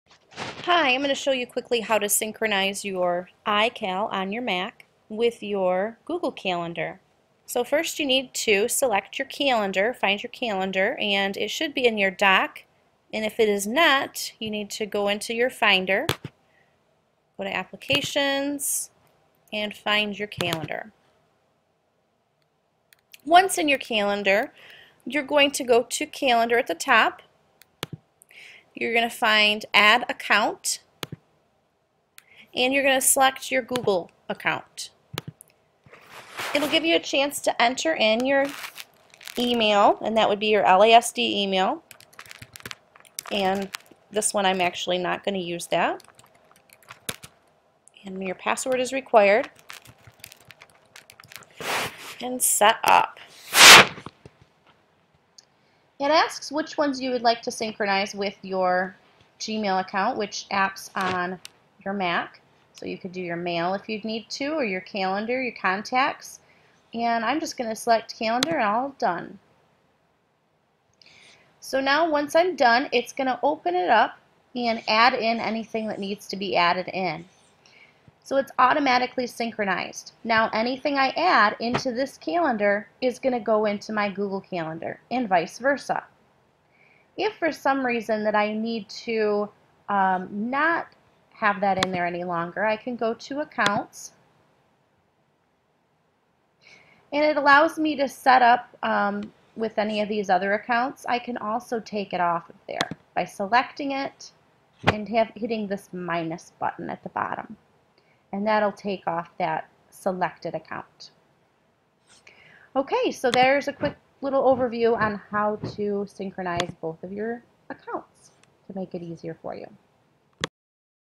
Sync iCal with Google Calendar
How to sync iCal on a Mac with Google Calendar.